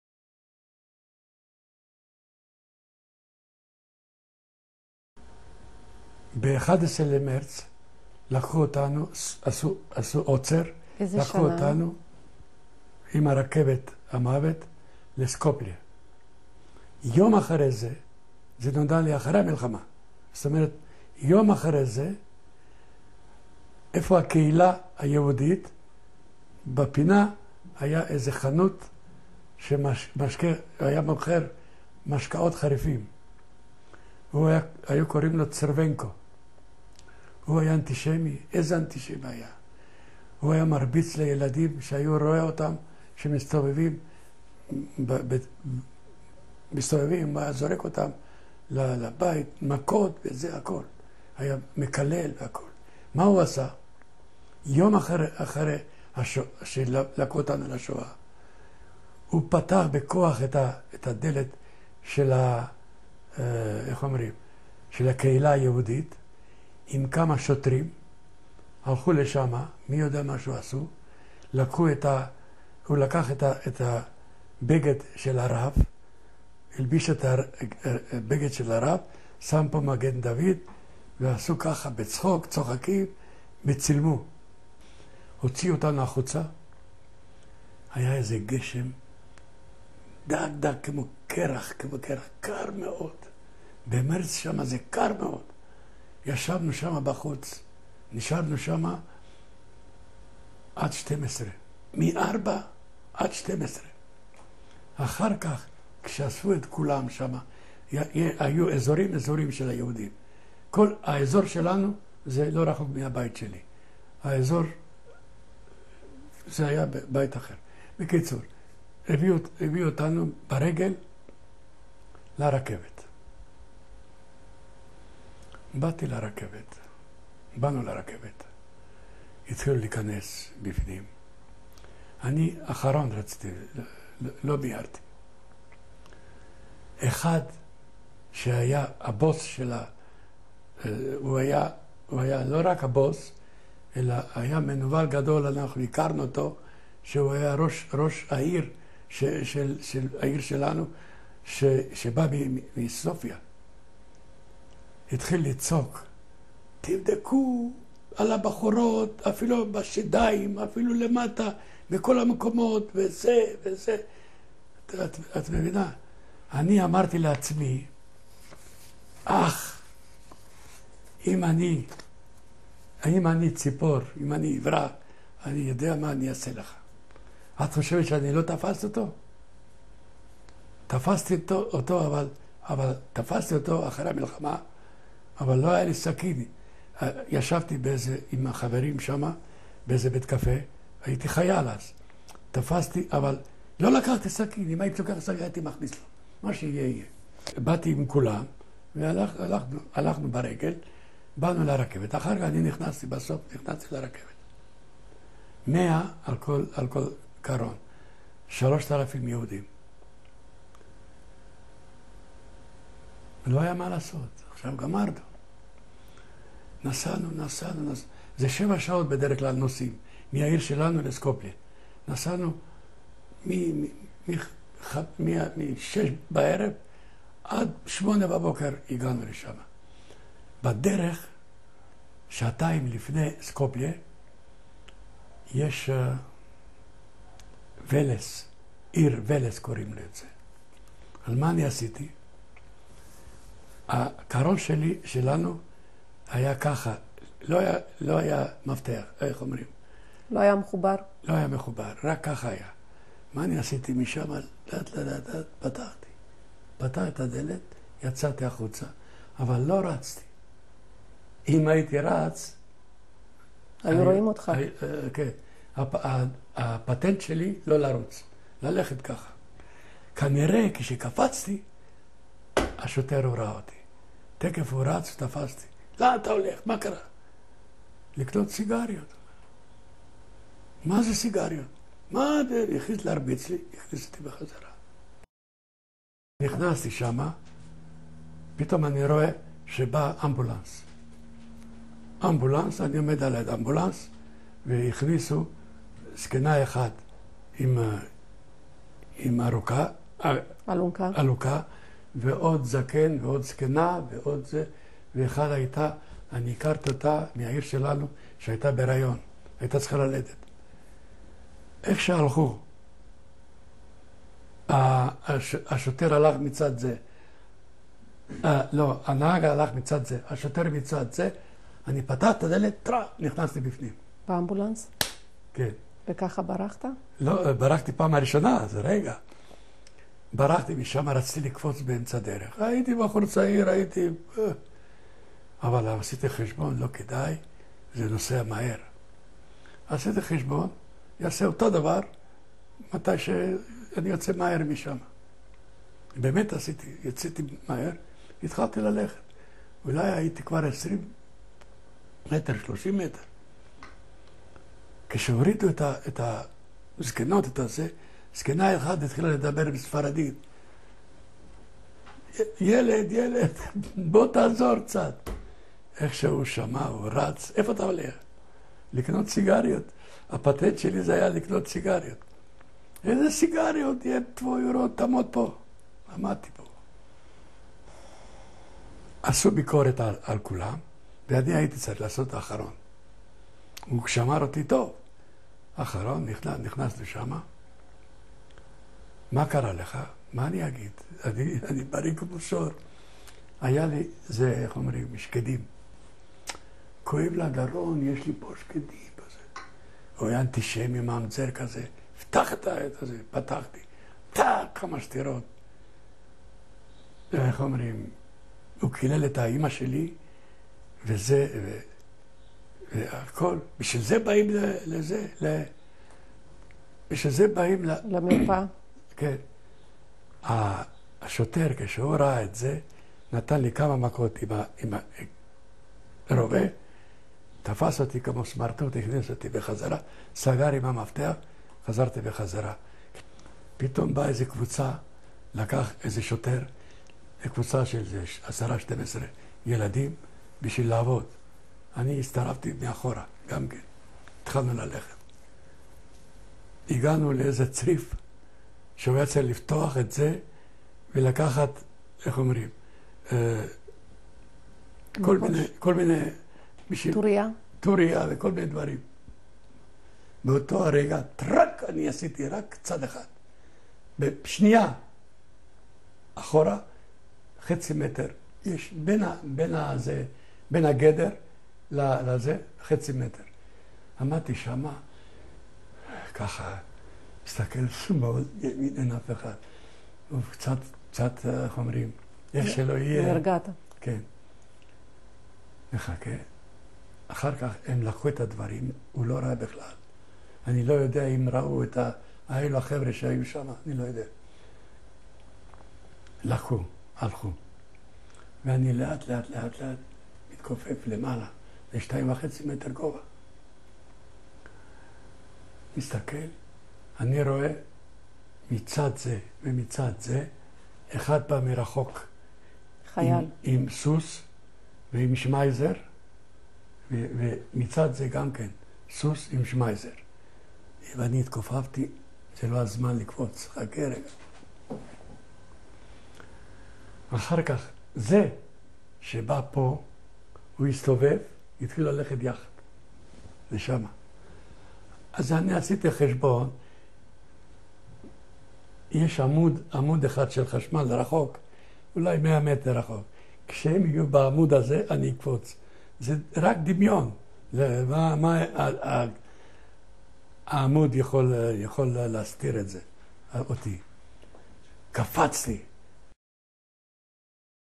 The End of the Jewish Community in Monastir, Macedonia
Holocaust survivor Niko Pardo describes the end of the Jewish community of Monastir, Macedonia, and how he escaped from the Bulgarian authorities.

This exhibition was prepared with the support of The Conference on Jewish Material Claims Against Germany